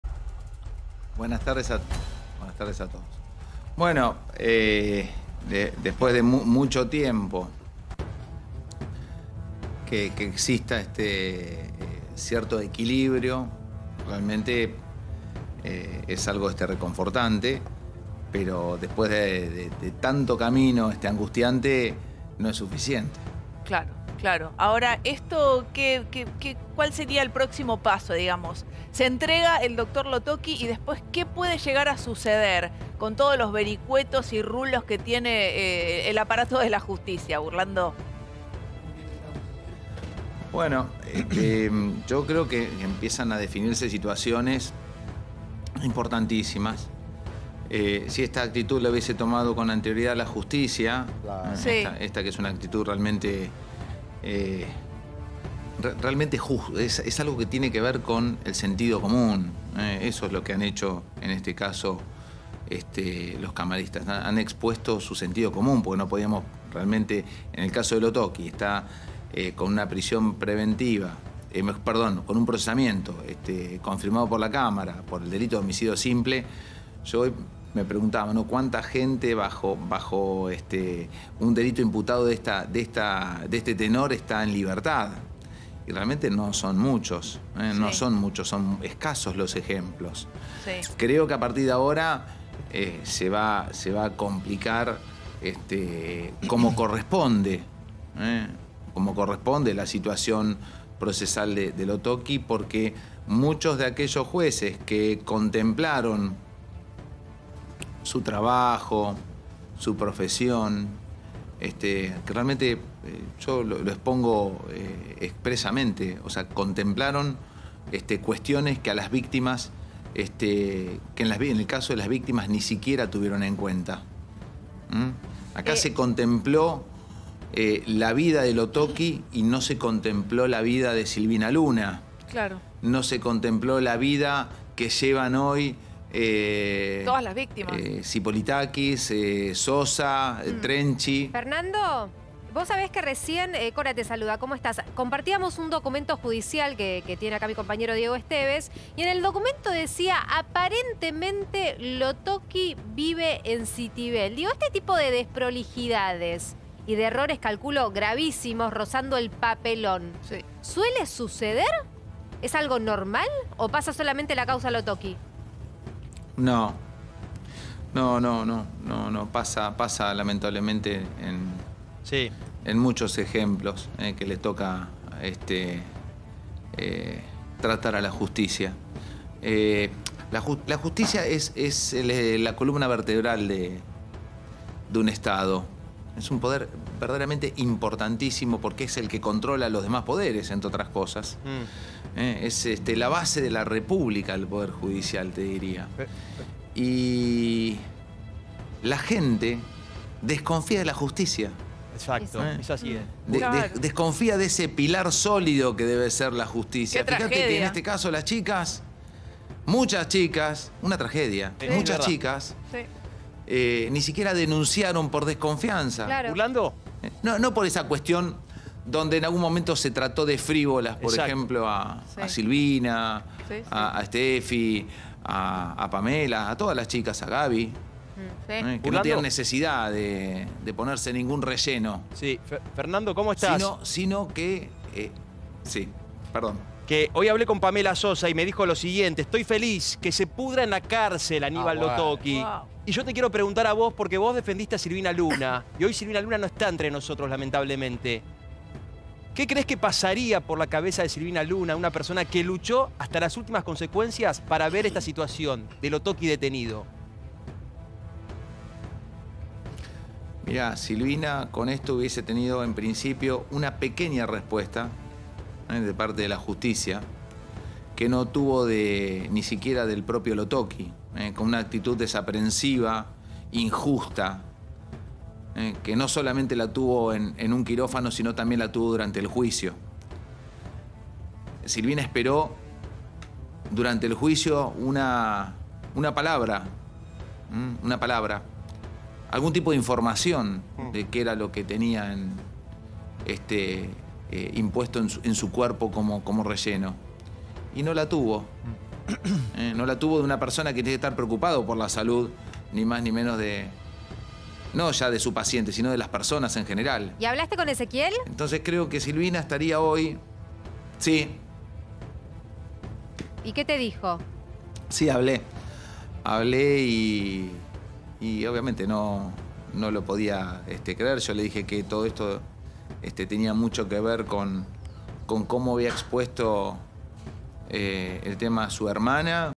La palabra de Fernando Burlando, sobre la detención de Lotocki
🔴 La palabra de Fernando Burlando, tras el pedido de detención de Aníbal Lotocki

💬 “Es reconfortante después de tanto tiempo”.